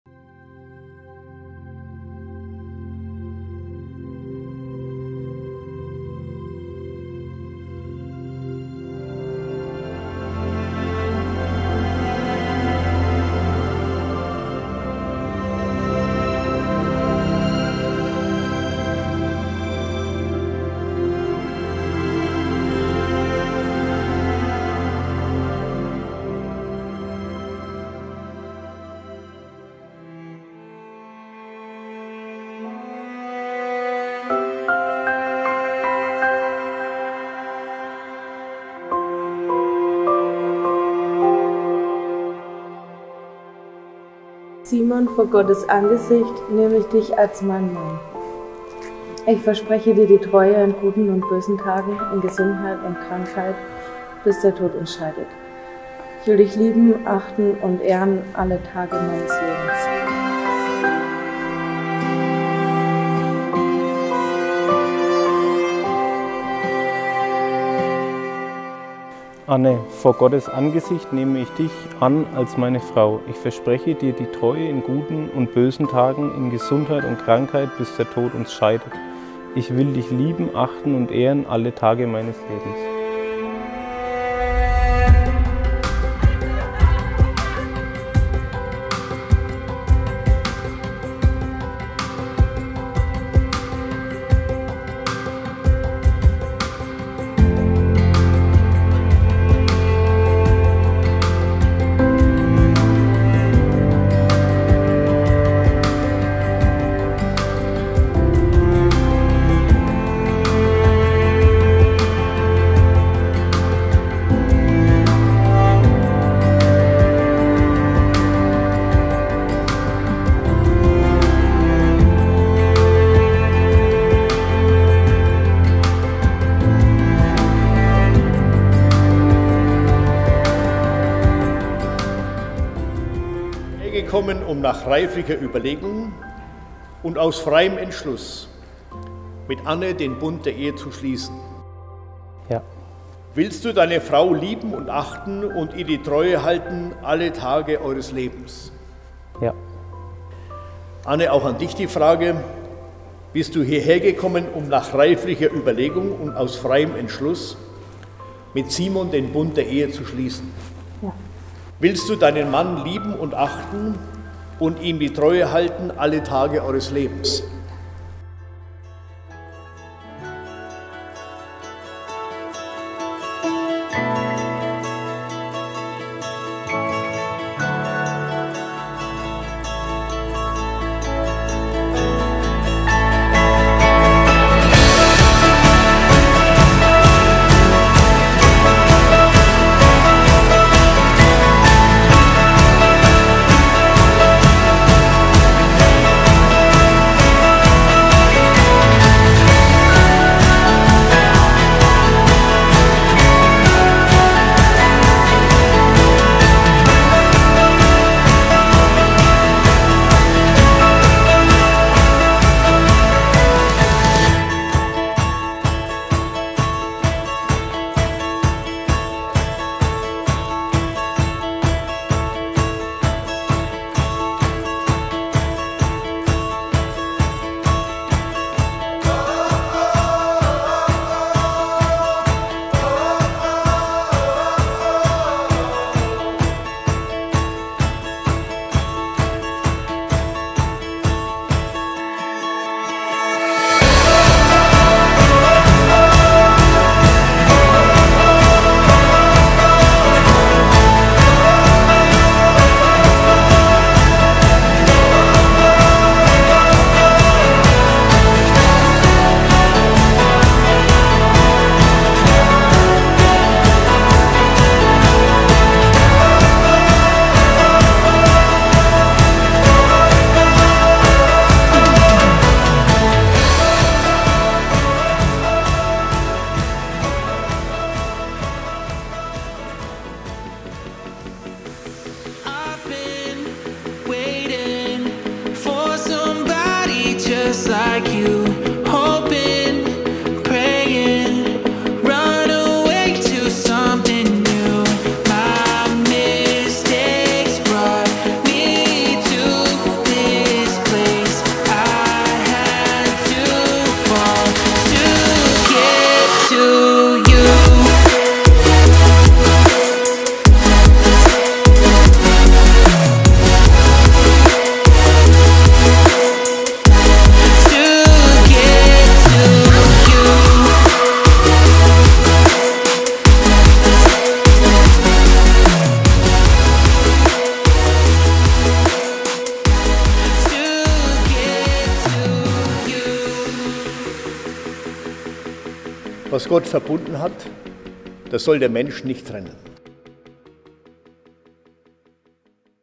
Hochzeitsvideo Bamberg - Anne & Simon
Eine Moody-Wedding, eine Hochzeit in schwarz ? So kann eine Hochzeit auch sein ?
Ja das kann sie ! Wir haben Anne & Simons großen Tag begleitet und damit dieses bezaubernde Hochzeitsvideo erstellt und das Konzept ihrer Hochzeit war das einer Moody-Wedding. Wir waren dabei vom Getting Ready bis zum Ende. Edel und in königlicher Pracht haben die zwei ihre Traumhochzeit im Residenzschloss in Bamberg gefeiert. Die Deko, die Gestaltung - alles in schwarz bzw. dunklen Farben. Und ja selbst wir als professionelle Hochzeitsdienstleister in Videografie und Fotografie können sagen, dass ist eine super und grandiose Idee. Die zwei haben von uns ihren Film mit Tonaufnahmen erhalten und wir können jedem kommenden Brautpaar nur empfehlen: Sagt euch etwas, auch wenn es nur ein paar Sätze sind, aber das macht für euch den Film tausendmal hochwertiger und ihr könnt somit die Gefühle nochmals von diesem Tag nachempfinden.

Wir wünschen euch viel Spaß beim Anschauen.